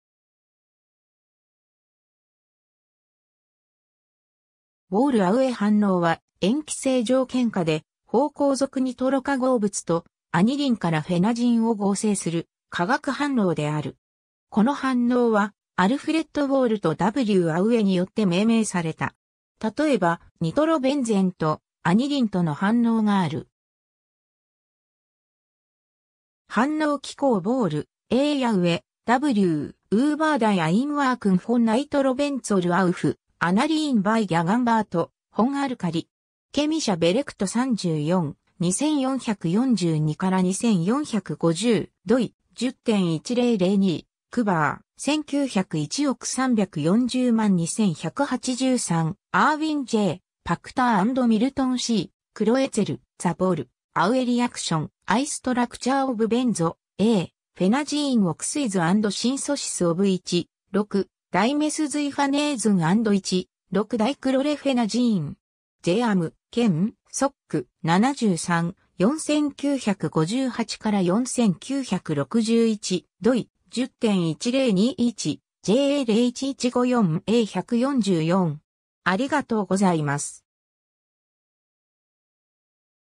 ウォール・アウエ反応
ウォール・アウエ反応（Wohl-Aue reaction）は、塩基性条件下で芳香族ニトロ化合物とアニリンからフェナジンを合成する化学反応である。
この反応は、アルフレッド・ウォールとW・アウエによって命名された。
例えば、ニトロベンゼンとアニリンとの反応がある。
 反応機構 ^ Wohl, A.; Aue, W. (1901). “Über die Einwirkung von Nitrobenzol auf Anilin bei Gegenwart von Alkali”. Chemische Berichte 34: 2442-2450. doi:10.1002/cber.190103402183. ^ Irwin J. Pachter and Milton C. Kloetzel (1951). “The Wohl-Aue Reaction. I. Structure of Benzo [a] phenazine Oxides and Syntheses of 1,6-Dimethoxyphenazine and 1,6-Dichlorophenazine”. J. Am. Chem. Soc. 73 (10): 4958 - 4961. doi:10.1021/ja01154a144.